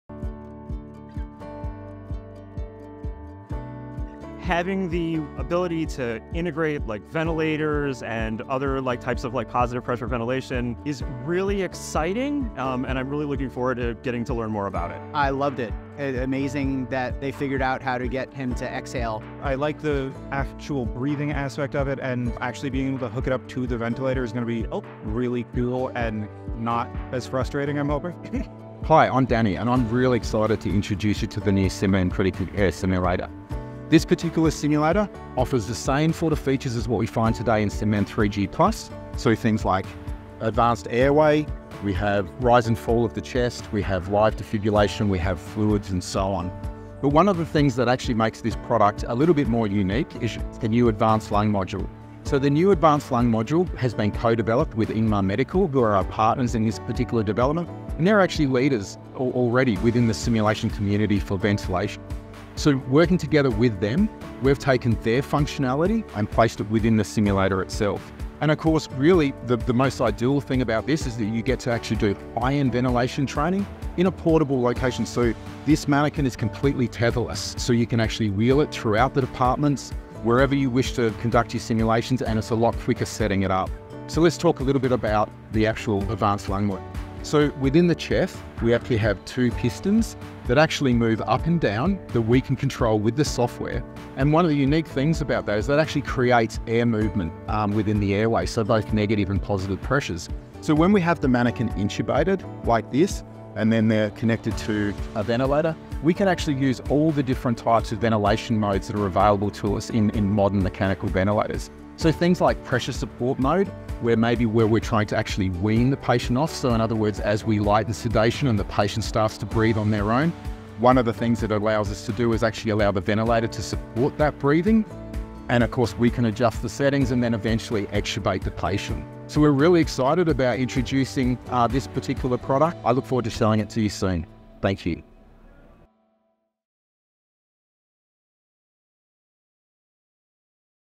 SimMan Critical Care Overview
SimMan Critical Care is an advanced respiratory and critical care simulator offering a full range of resistance, compliance, and patient effort features, ensuring healthcare teams are prepared for the challenges of high-acuity cases.